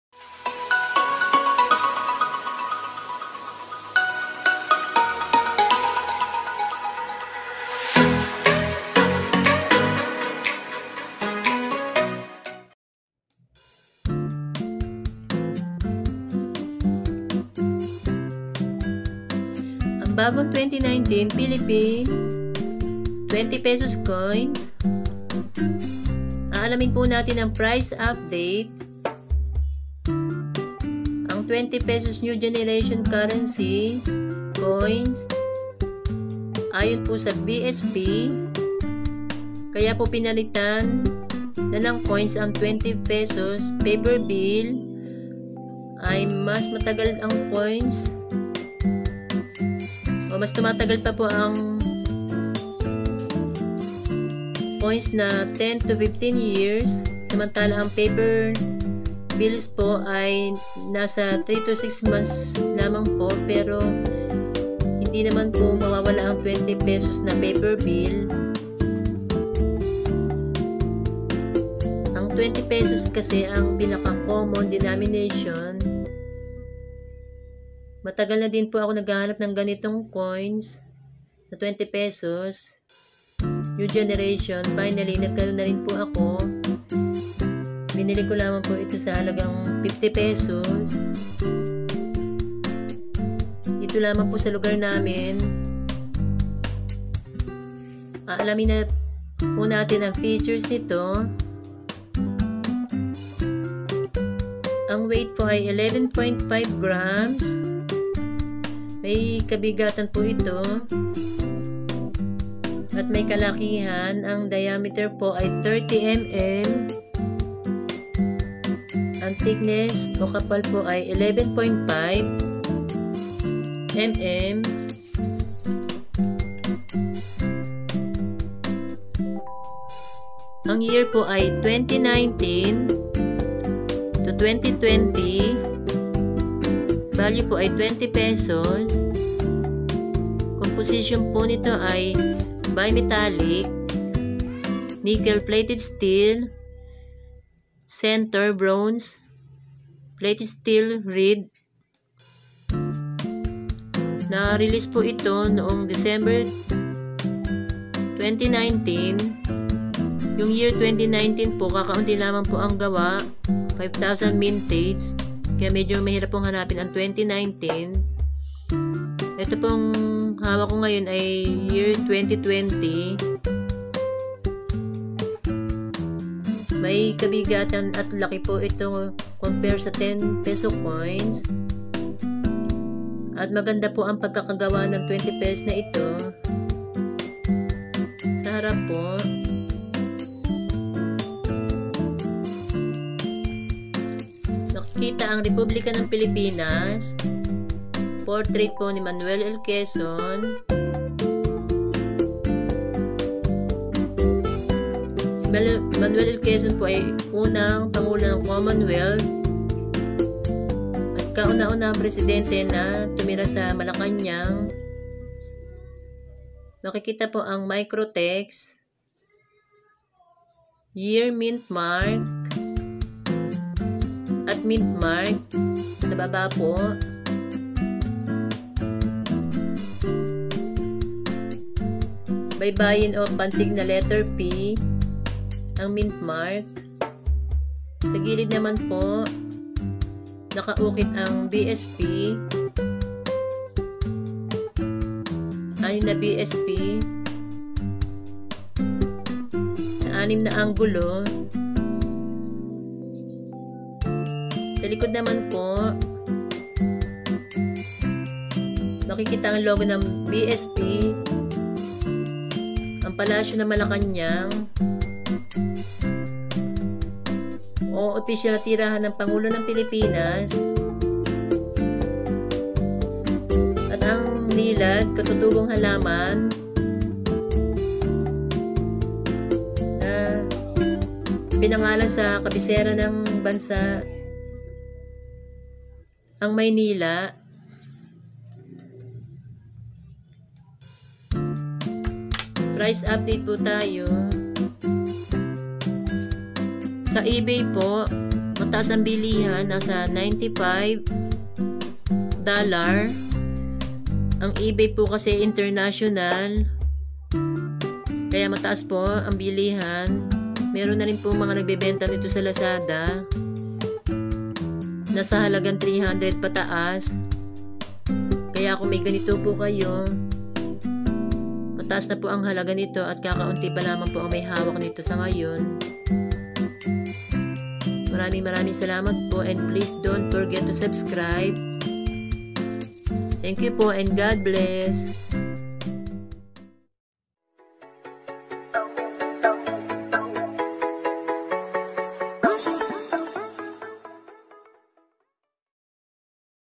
Bagong 20 Peso ng Pilipinas | NGC may Value na | Collection 2020
Ang mga pera na ito ay bago ginagamit ito sa panahon ngayon, nagiging mataas ang halaga nito sa mga COIN COLLECTORS na nais mapuno ang kanilang mga baryang kinokolekta.

Mag comment na rin kayo kung mayroon kayong ganiting COINS OR OLD COINS para makita ng mga COINS COLLECTORS na nais BUMILI ng inyong mga COINS OR OLD COINS.